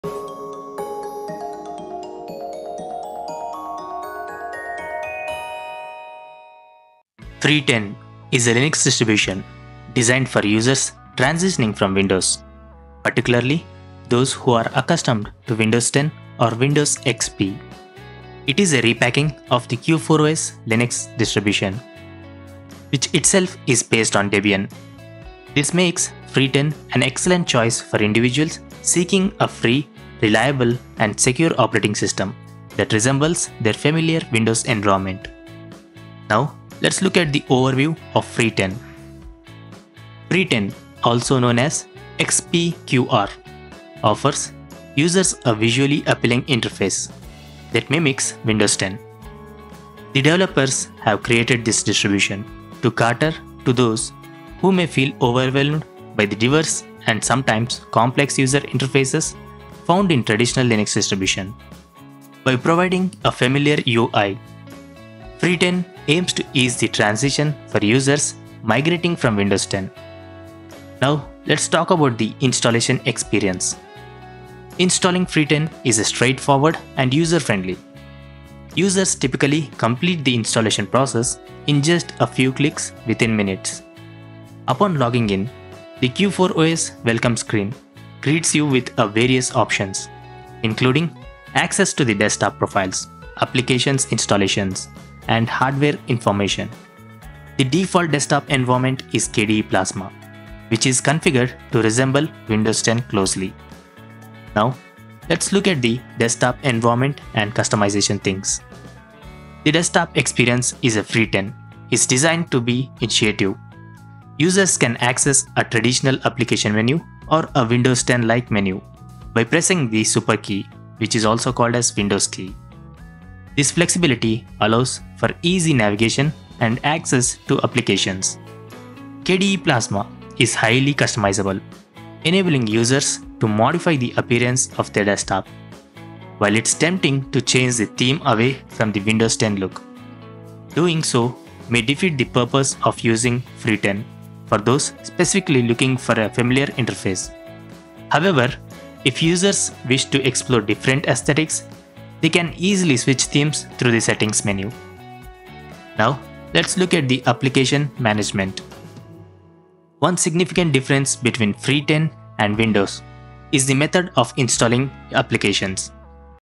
Free10 Linux: The Ultimate Windows 10 Alternative!
Tired of Windows but not ready to give up that familiar feel? Meet Free10 Linux—a free, reliable distro built on Debian and Q40S that delivers a Windows 10-like experience! In this video, we dive into its seamless installation, intuitive KDE Plasma desktop (complete with a Windows-style menu), and powerful customization options that make transitioning from Windows a breeze. Whether you're a Linux newbie or a seasoned pro looking for a change, discover how Free10 Linux can revolutionize your workflow with security, speed, and style. Watch now and see why Free10 is the ultimate OS upgrade!